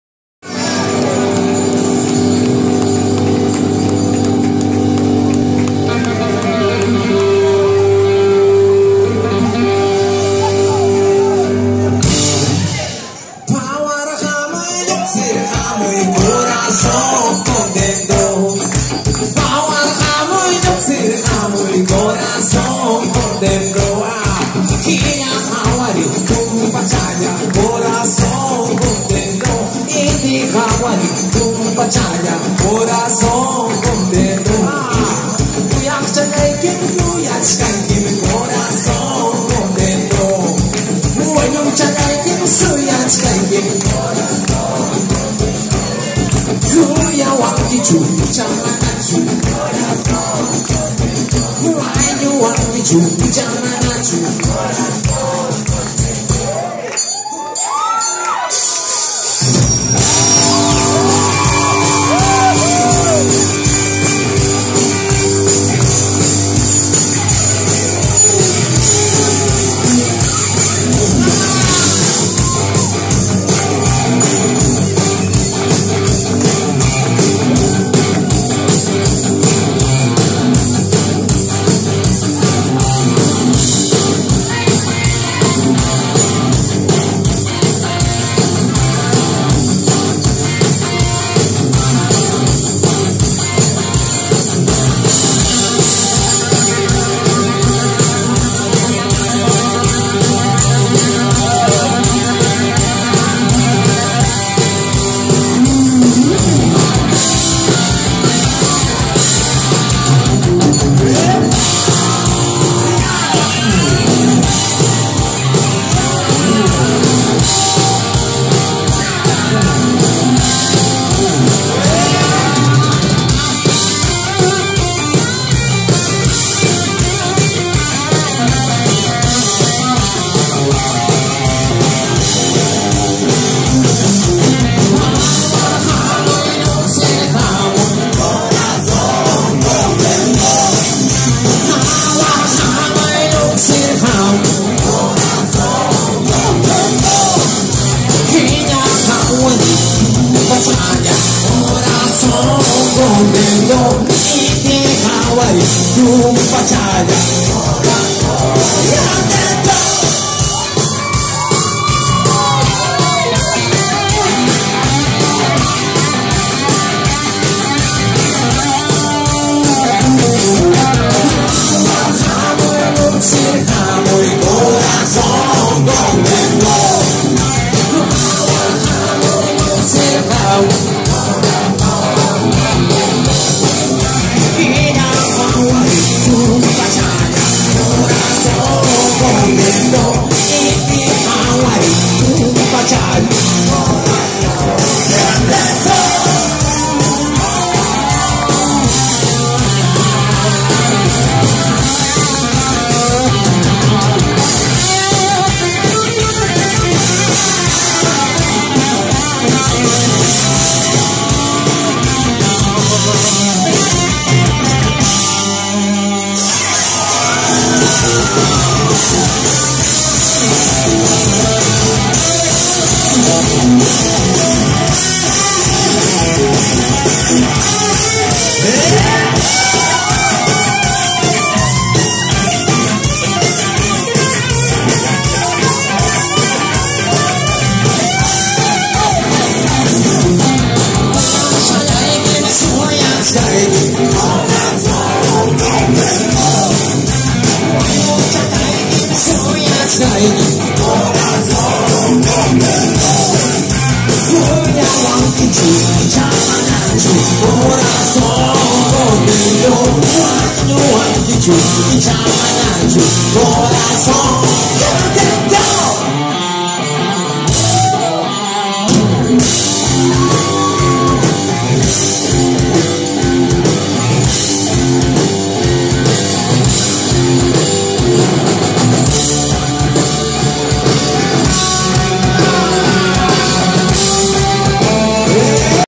UCHPA en La Noche Bar de Barranco #1- 2-24-17
El fantástico grupo de Uchpa cantando Rock y Blues en Barranco Lima. Uchpa en La Noche Bar de Barranco #1